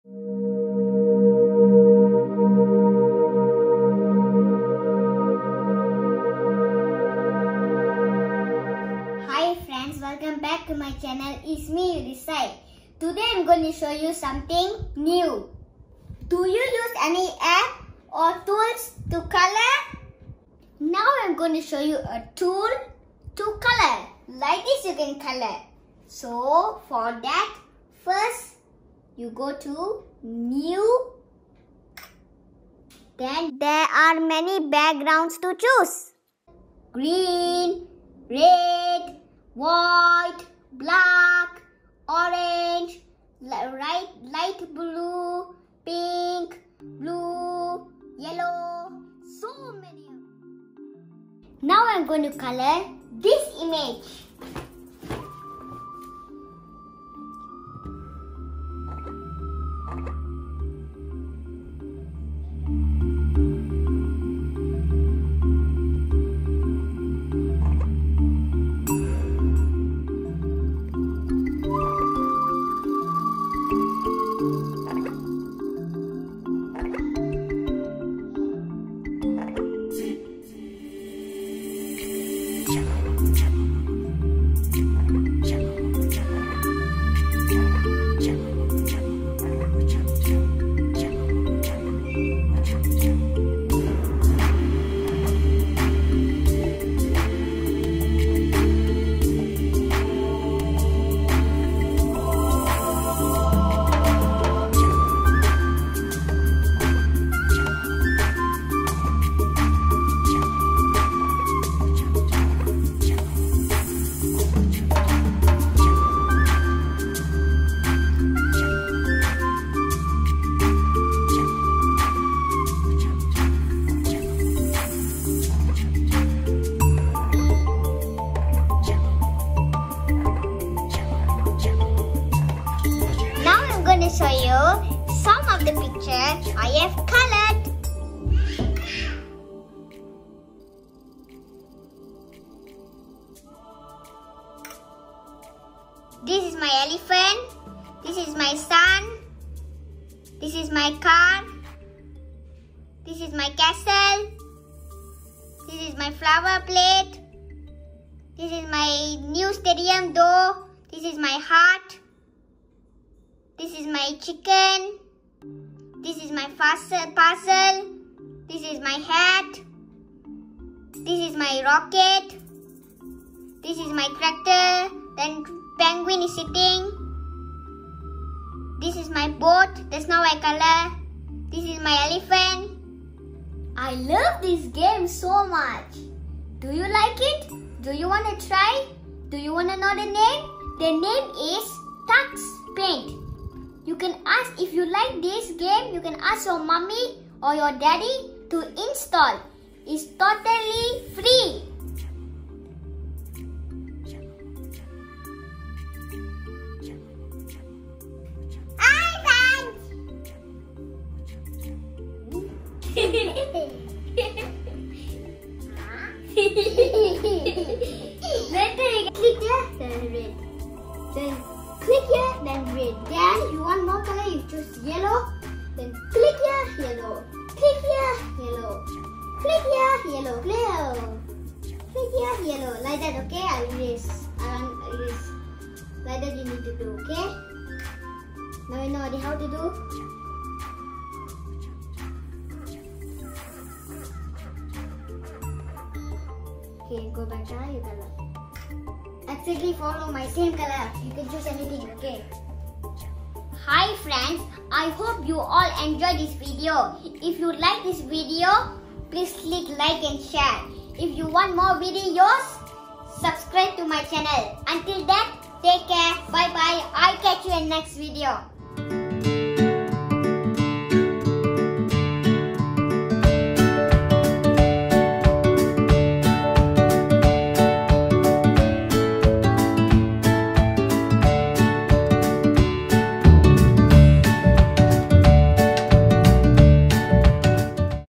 Tux paint for kids, preschool learning | educational videos | vídeos educativos
Kids free app to teach computer skills and drawing. All the parents and kids love this free app. Kids learn to color and drawing.